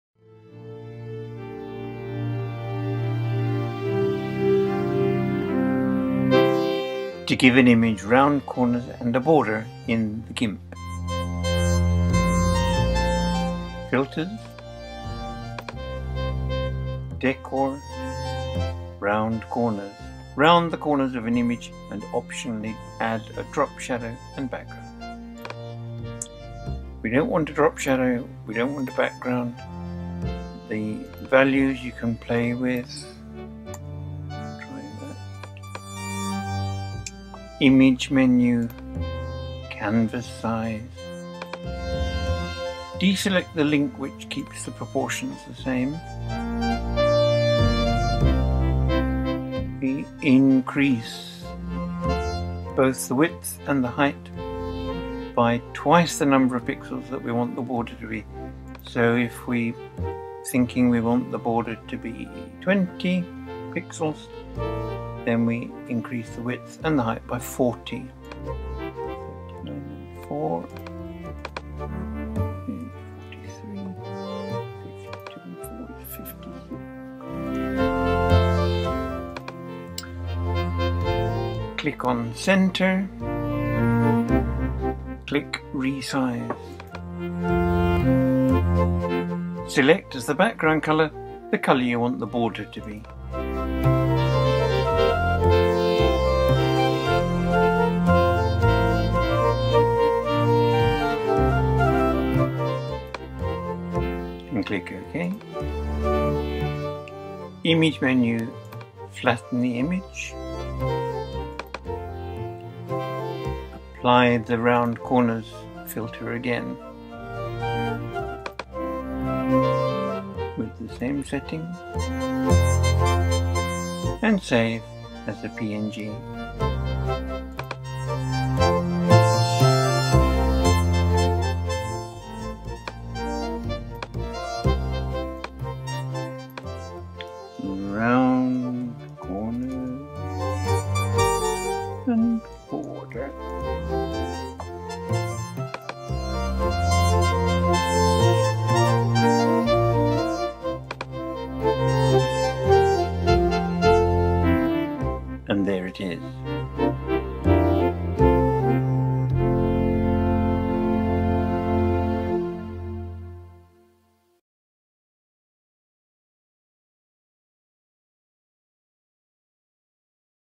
Round corners with the GIMP - 2 with a border
David Kettlewell shows how you can give a photo rounded corners and a rounded border in the GIMP (the free alternative to Photoshop, Gnu Image Manipulating Program) on Mac OS X; the music is also him, improvising on Swedish folk music on the accordion.